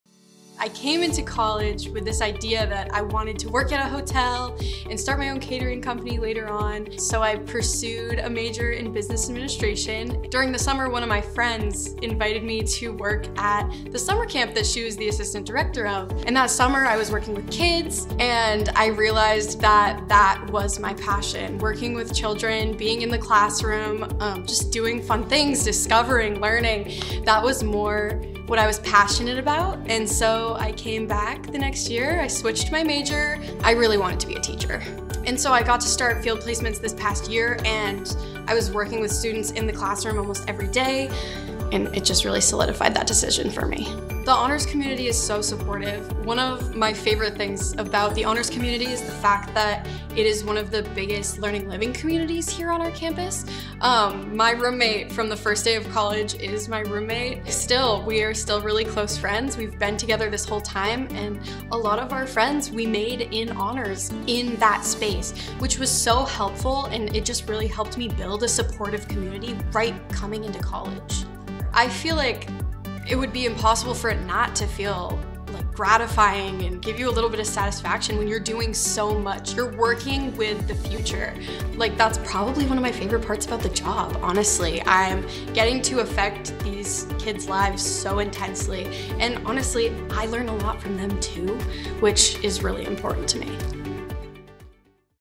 Morgan Inman | 2024 College of Education and Human Development Outstanding Graduate
Morgan Inman of Wales, Maine, is the Outstanding Graduating Student in the College of Education and Human Development. She is an elementary education major with a concentration in mathematics and a minor in business administration, she is also a member of the Honors College. During her time at UMaine, she produced a thesis for the Honors College titled “Sexuality Education In Central Maine High Schools: What’s Happening Now and What’s Changed In Two Decades.” The qualitative study examined sex education in central Maine high schools by interviewing teachers in those schools. Inman has also been involved with a local after-school and summer camp program for three years, acting as an assistant director for the program for the last two years. She is a Maine Top Scholar and recipient of the Galen Cole Family Foundation Teaching Scholarship. After graduating, Inman plans to enter the education field as a classroom teacher and pursue a master’s degree.

Become a Black Bear! 🐻